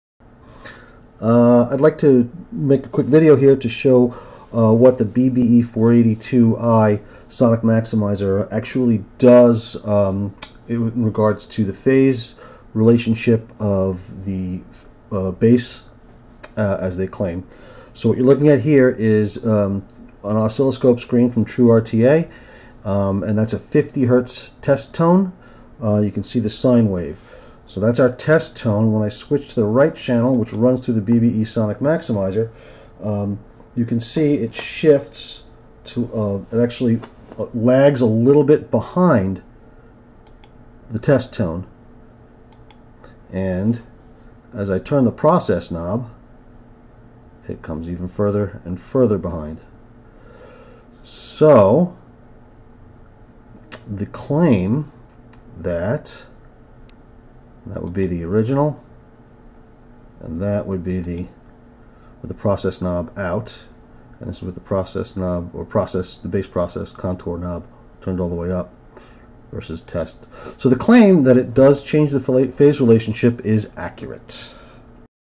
BBE Sonic Maximizer Phase Shift Measured.
BBE have been repeatedly bashed for this product, with claims of snake oil etc.  I have spent quite a bit of time with the unit and have thus far found every claim to be accurate, and measurable.   This is a quick vid showing how the phase of a 50hz test tone does indeed shift as you turn the bass contour knob.